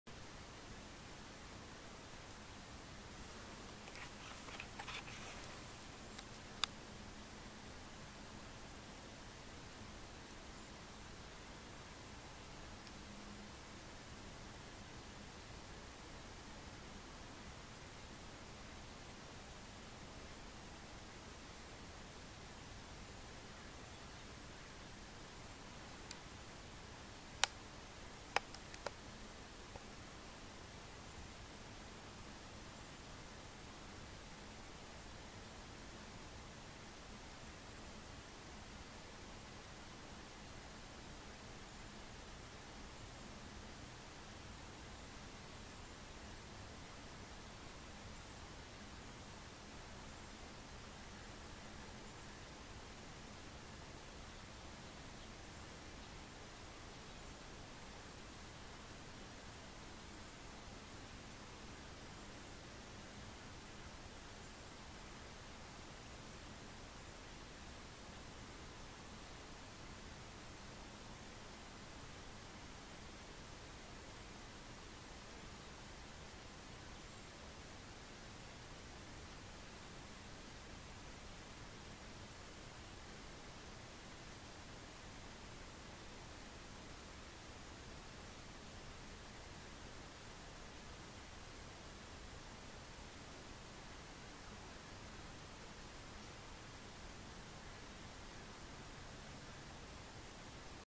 Born Free
The wild little tigress who lives in my back yard. She's playing with a petrified squirrel poop.